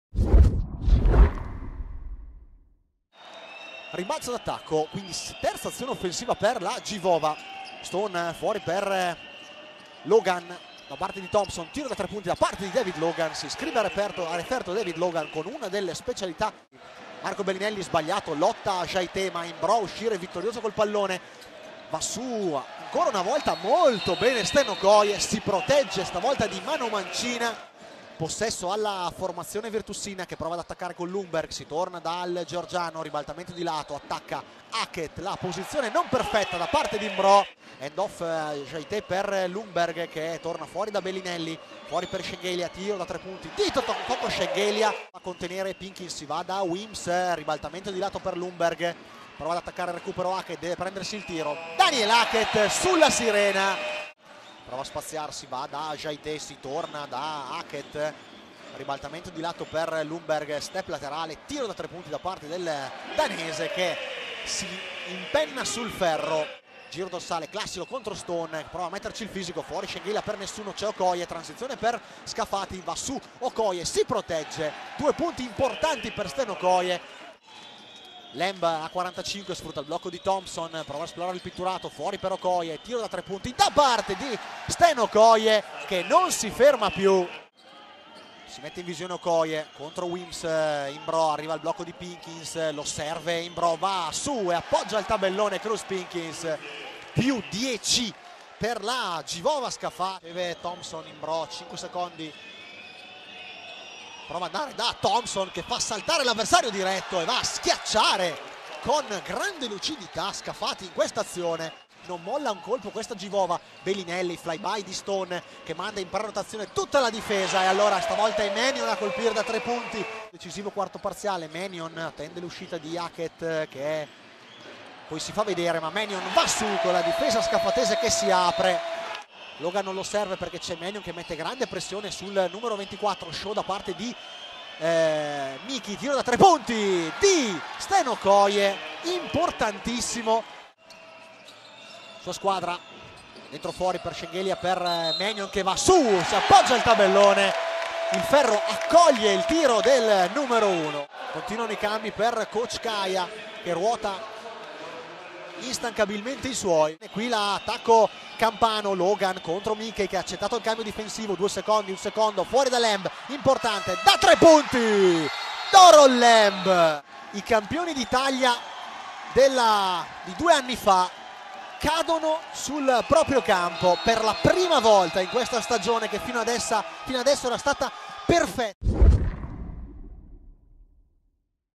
BOLOGNA - SCAFATI HIGHLIGHTS | LBA - 10^ Giornata
VIRTUS SEGAFREDO BOLOGNA - GIVOVA SCAFATI Highlights della partita
Le migliori azioni della sfida tra Bologna - Scafati per la 10^ giornata